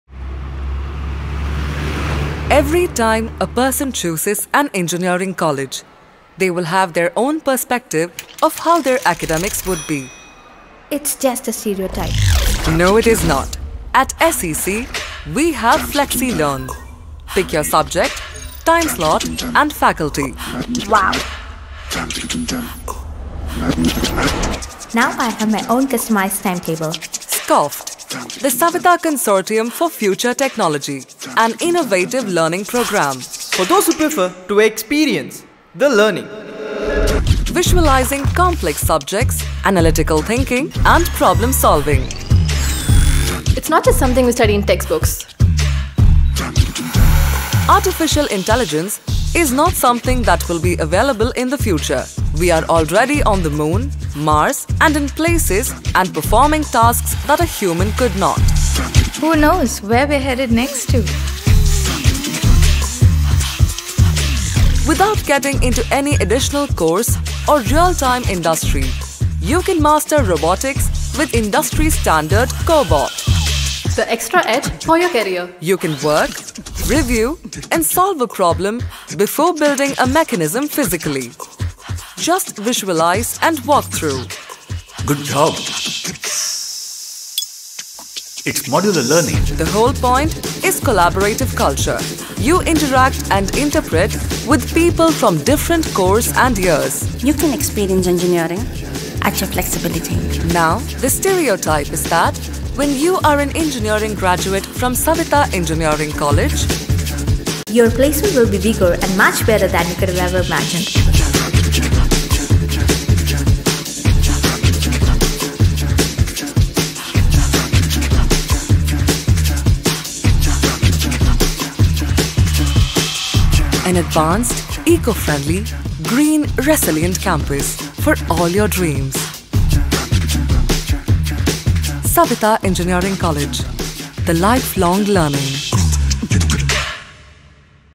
Embark on a future-engineering journey at Saveetha Engineering College (Chennai)
Unlock the future of innovation at Saveetha Engineering College. Dive into our Centres of Excellence with cutting-edge AR/VR, Blockchain, Cybersecurity, Robotics, Machine Learning, IoT, 3D Printing, PCB Prototyping & SMT Assembly and Embedded Design Innovation Design Center and more. Shape your engineering dreams with us!